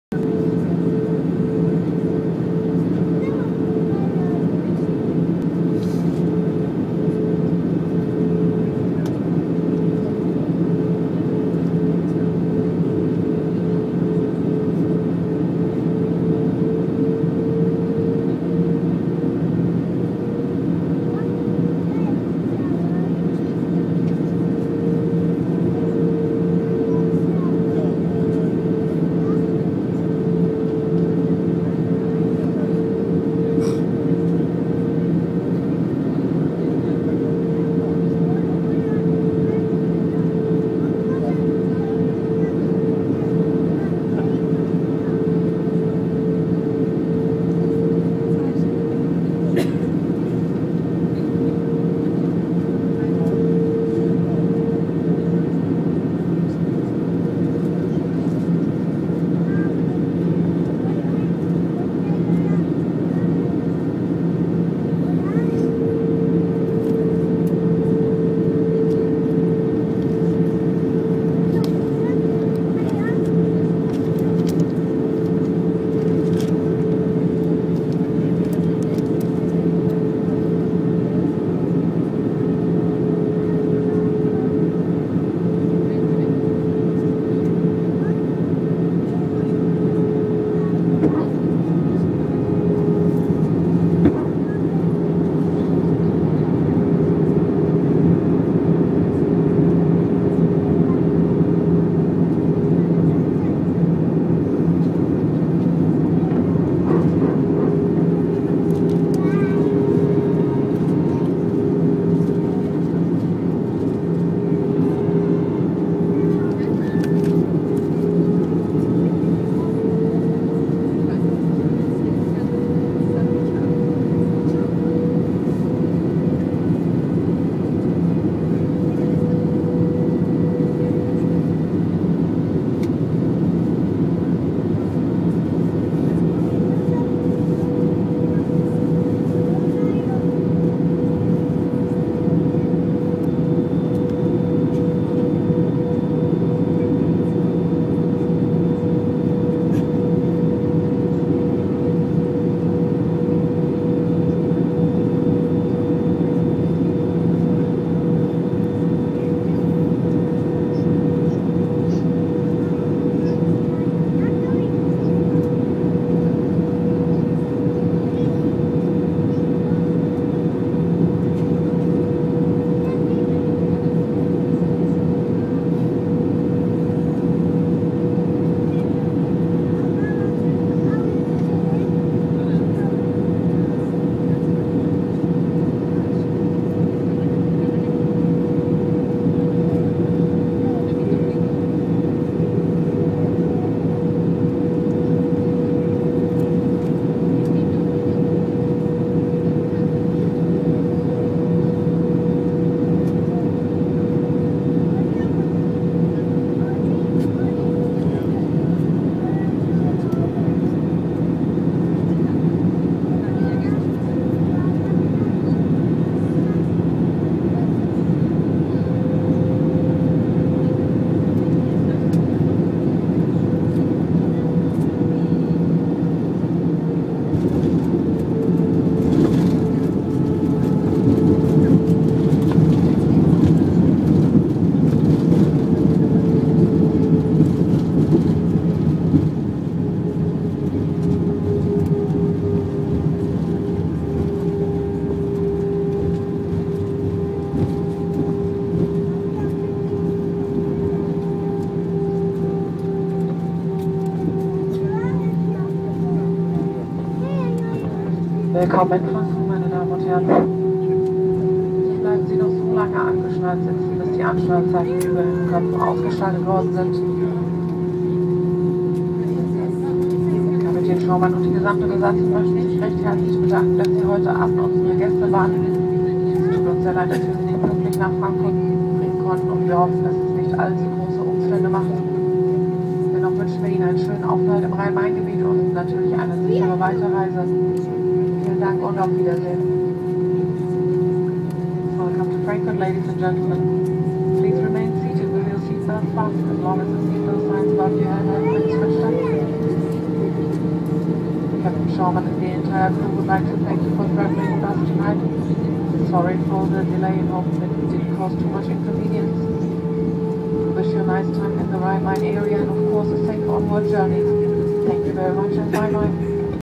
Lufthansa LH193 TXL Berlin to FRA Frankfurt - Short Final & Touchdown - 28.08.2010
Last 5.16 on flight LH193 from Berlin to Frankfur am /Main, including touchdown.